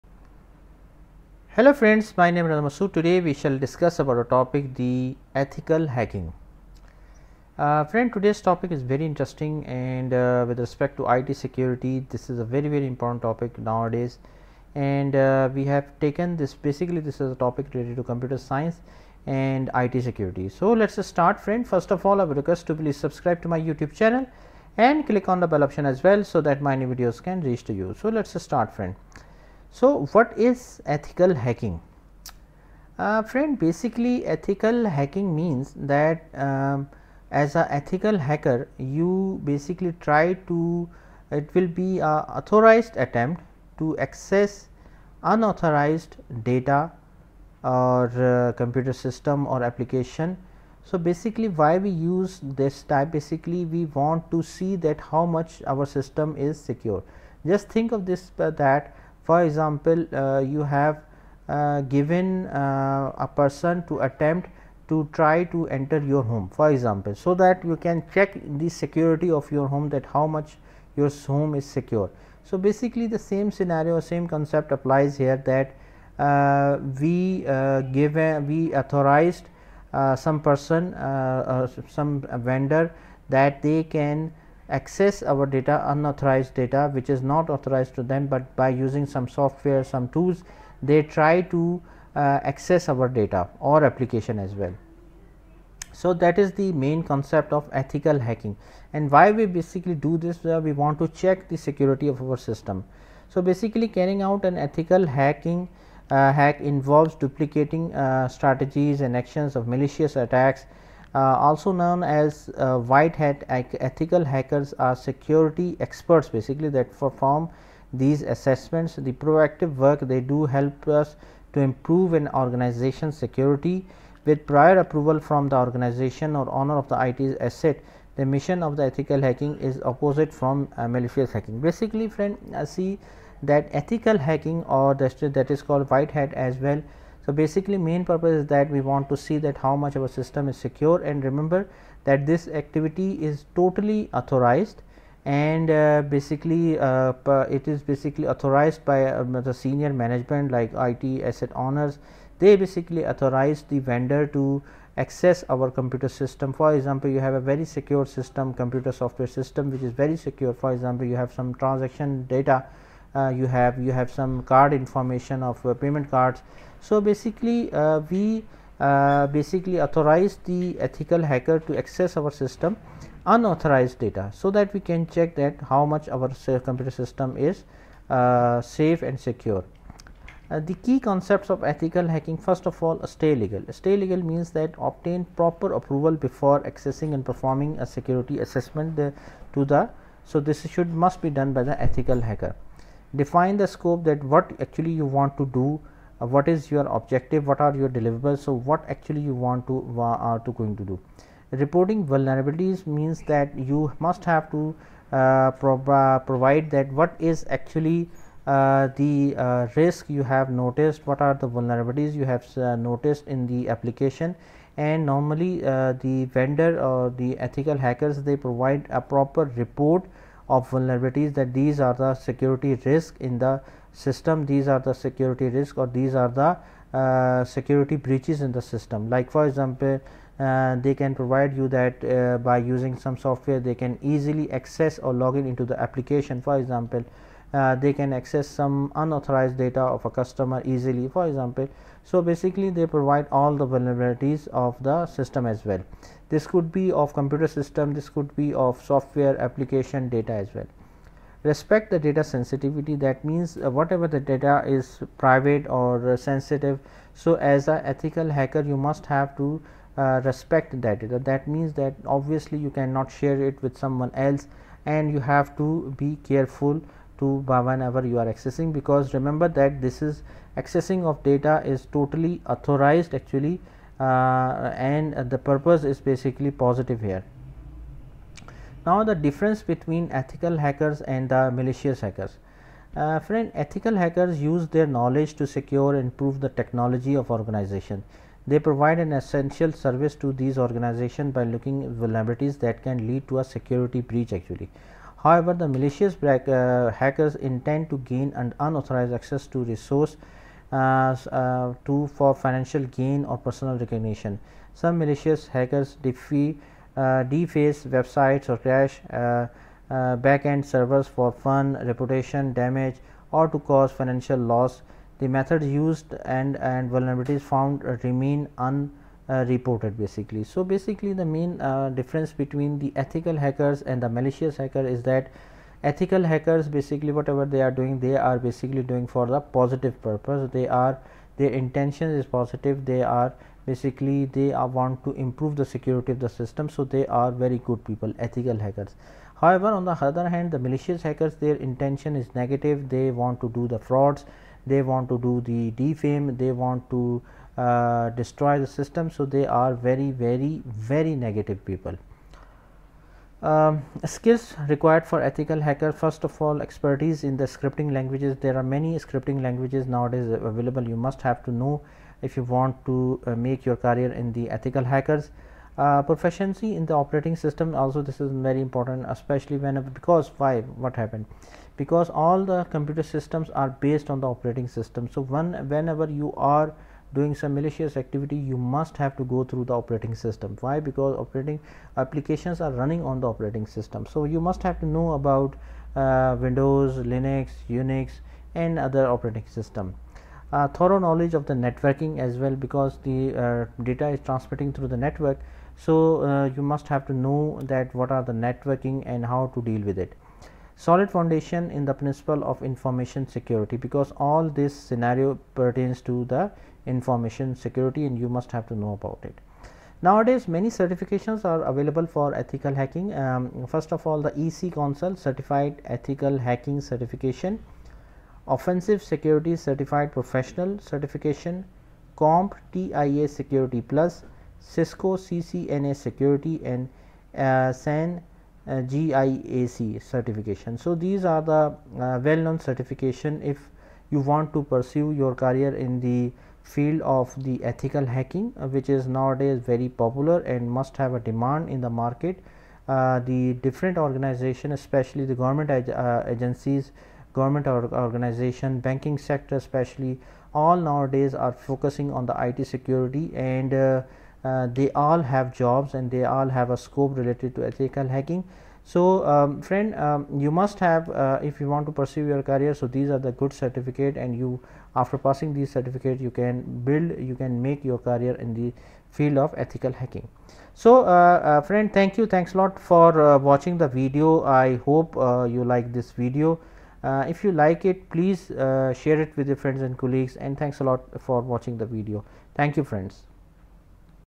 What is Ethical Hacking
Ethical hacking involves an authorized attempt to gain unauthorized access to a computer system, application, or data. Carrying out an ethical hack involves duplicating strategies and actions of malicious attackers. This practice helps to identify security vulnerabilities which can then be resolved before a malicious attacker has the opportunity to exploit them.

Also known as “white hats,” ethical hackers are security experts that perform these assessments. The proactive work they do helps to improve an organization’s security posture. With prior approval from the organization or owner of the IT asset, the mission of ethical hacking is opposite from malicious hacking.